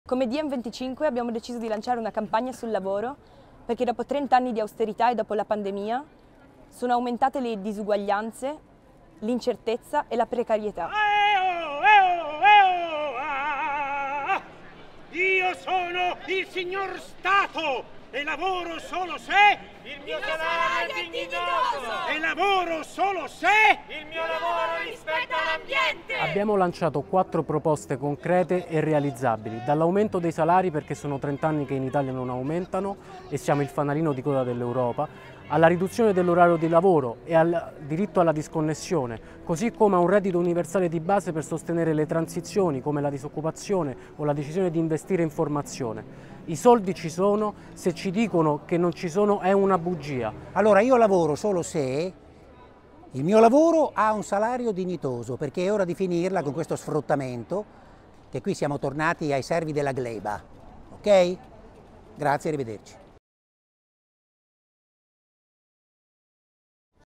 DiEMers in Italy stage flashmob to launch Lavoro Se, a campaign to reclaim labour rights in Italy
On Thursday April 28, DiEMers in Italy staged a flashmob to launch Lavoro Se, a campaign to reclaim labour rights in Italy.

Live in Italy? Visit lavoro-se.it and join us in taking a few, simple online actions to protect labour rights in Italy.

The flashmob, organised together with performers from the Constitutional Circus, took place at Piazza della Rotonda in Rome.